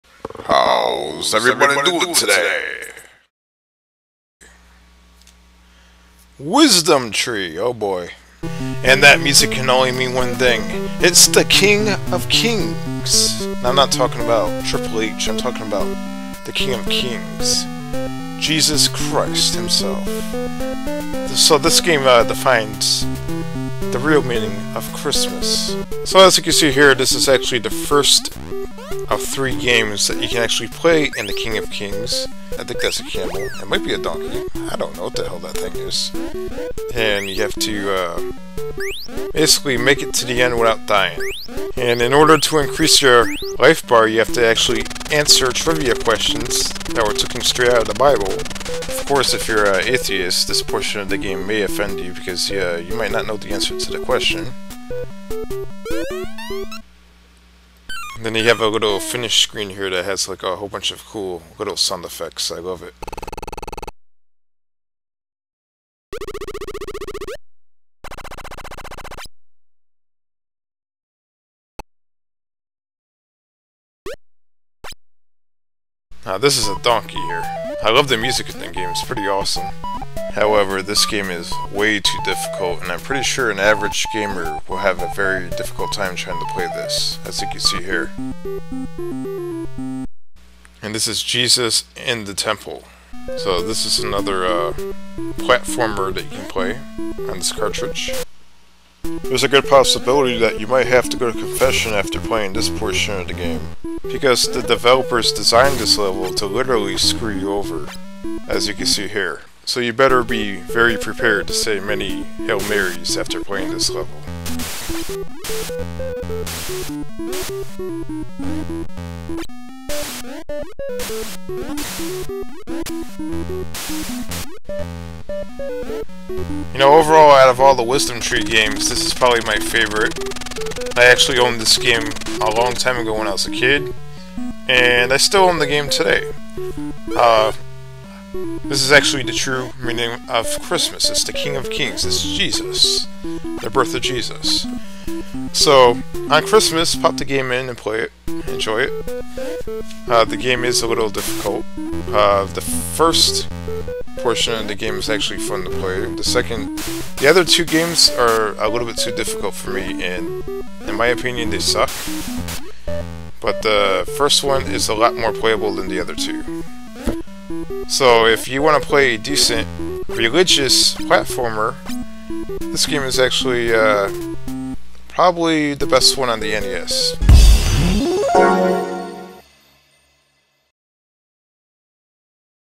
NES - King of Kings (1 Minute Review)
This is an extended special Christmas review of the religious classic, King of Kings on the NES.